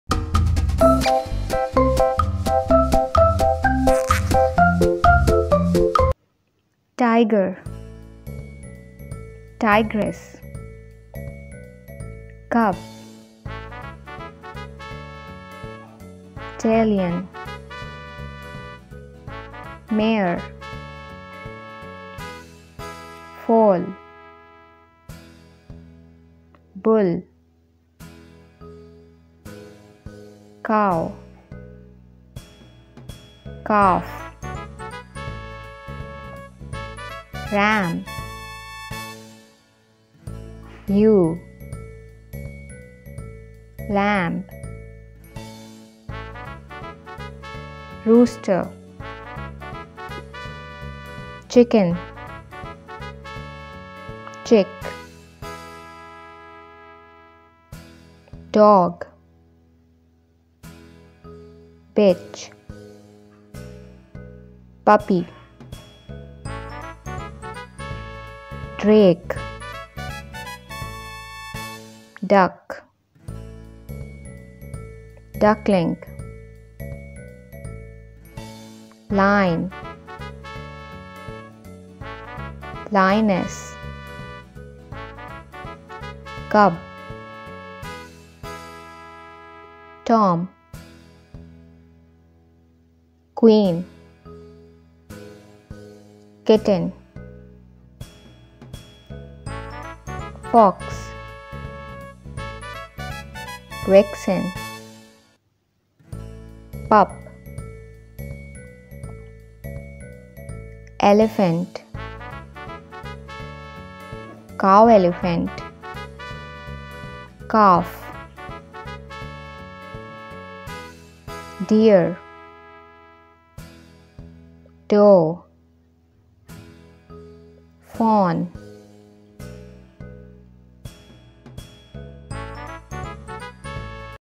Baby Animals| Animals and Their Young Ones
Hey Vocabaholics,

This video is a fun learning one, this video will teach the kids about the names of baby animals and what their parents are called, animals and their young ones are presented in picture form in this video, hope you and your kid will enjoy this list of cute baby animals.

A baby tiger is called a cub
A baby horse is called a foal
A baby bull is called a calf
A baby sheep is called a lamb
A baby hen is called a chick
A baby dog is called a puppy
A baby duck is called a duckling
A baby lion is called a cub
A baby cat is called a kitten
A baby fox is called a pup
A baby elephant is called a calf
A baby deer is called a fawn

Stock material:
Hope you will enjoy watching our videos.

Be a Happy Vocabaholic Learner!🤓

Keep Watching and Learning!